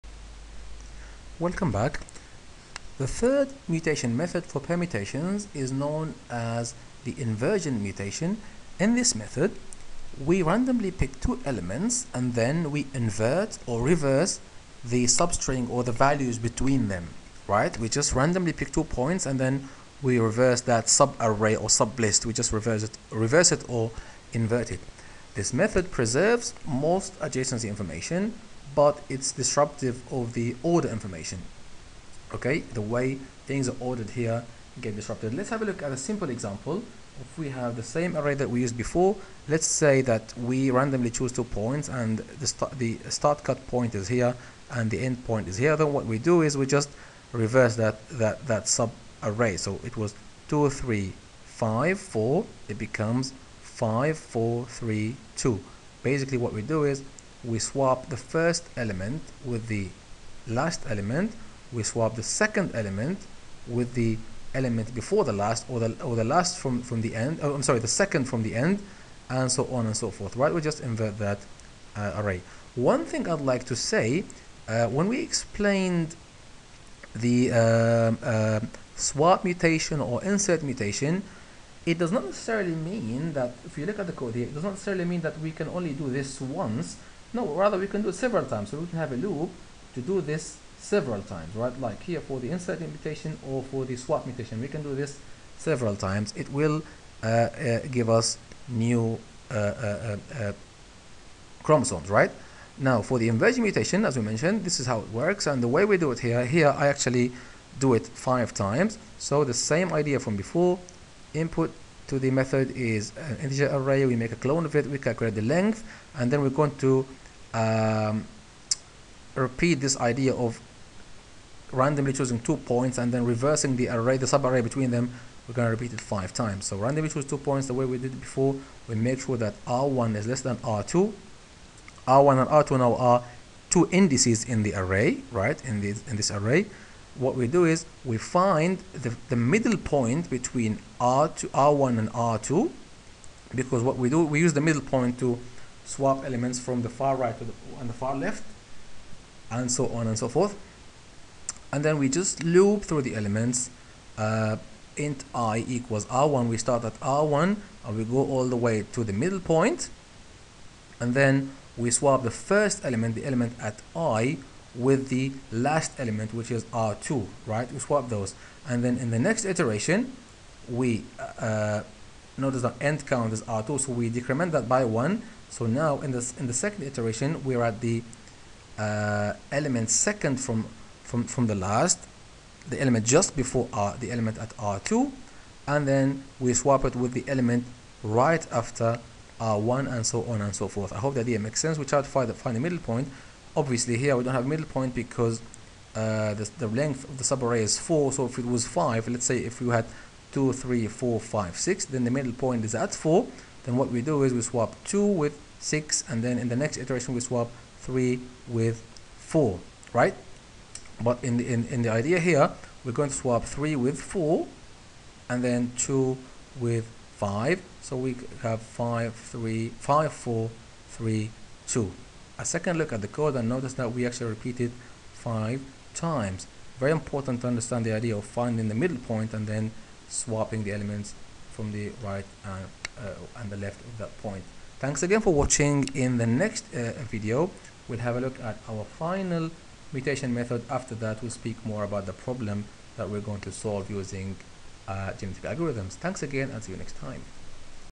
Genetic Algorithms 24/30: Inversion Mutation with Java Implementation
In this series I give a practical introduction to genetic algorithms